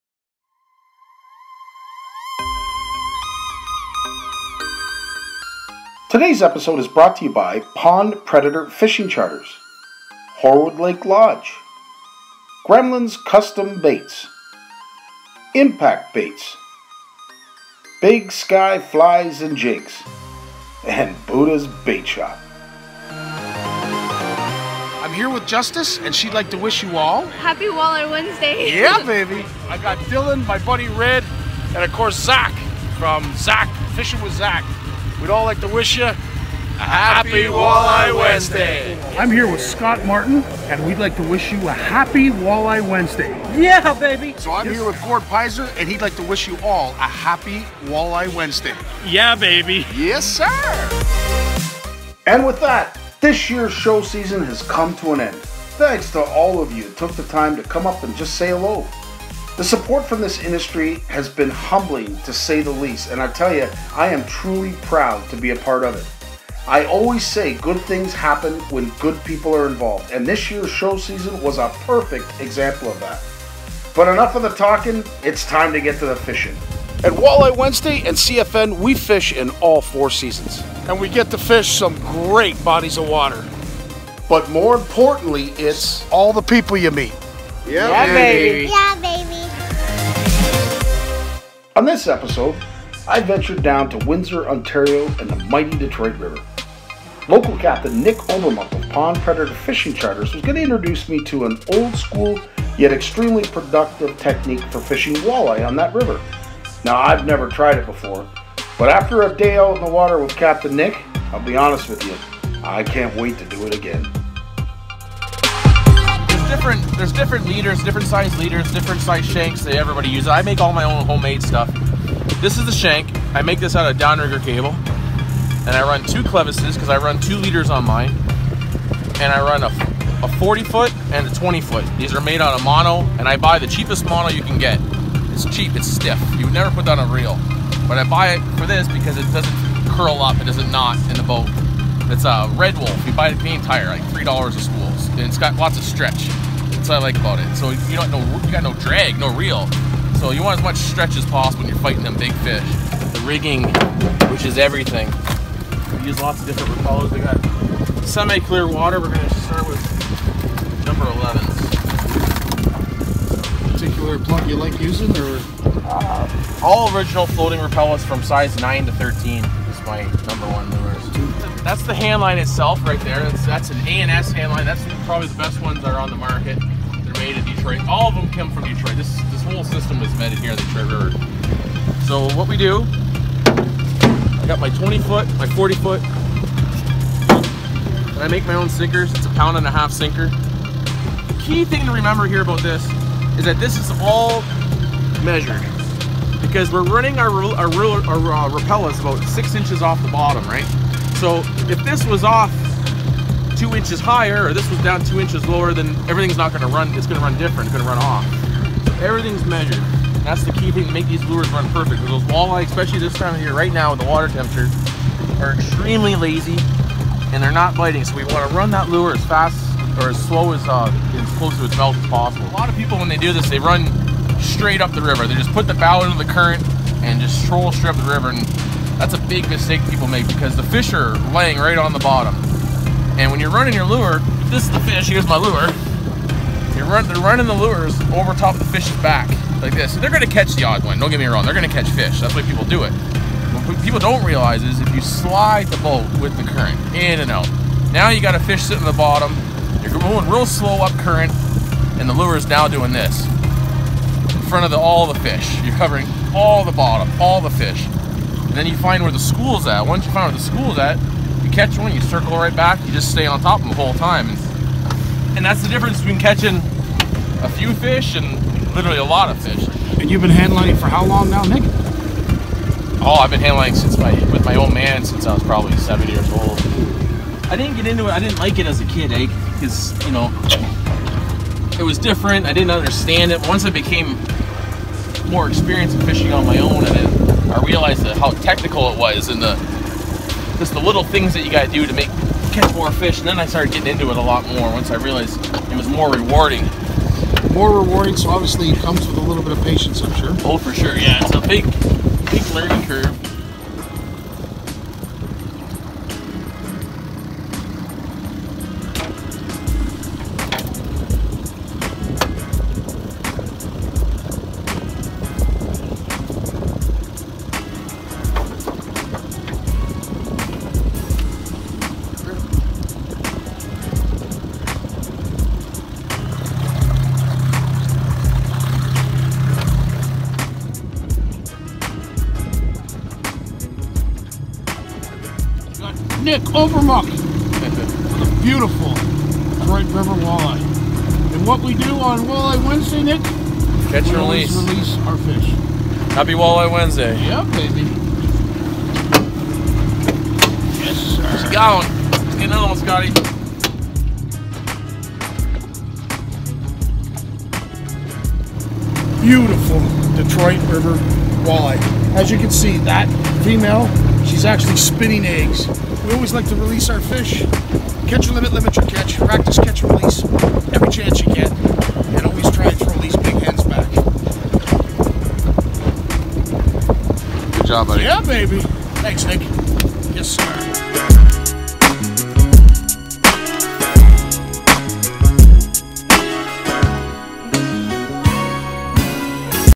Walleye Wednesday Episode 105 - feat Pond Predator Fishing Charters
🇨🇦 Happy Walleye Wednesday 🇨🇦 - 105th Edition

Featuring:  🏆  Handling For Walleye on the Detroit River  🏆

The “grassroots” of Walleye Wednesday has always been about educating Anglers on the importance of Catch and Release.

However, each week it sometimes morphs into something a little bit more.

In this episode I focus on this year’s past fishing show season.  It was truly a lot of fun!!!
...and thanks to all who simply said, “hello”.

Also, I travel down to the Detroit River in Windsor, to try my hand at “hand-lining”. Captain Nick Oberemok of PondPredatorFishingCharters is a fantastic guide who has learned how effective this application can be for river Anglers. It certainly was on this day. Thanks again Nick!!!

Also, many thanks to Scott Martin of the ScottMartinChallenge, Gordon G Pyzer, Dylan K Moore, Zak Prenger, Red Andal, and of course Justice for their WalleyeWednesday greetings.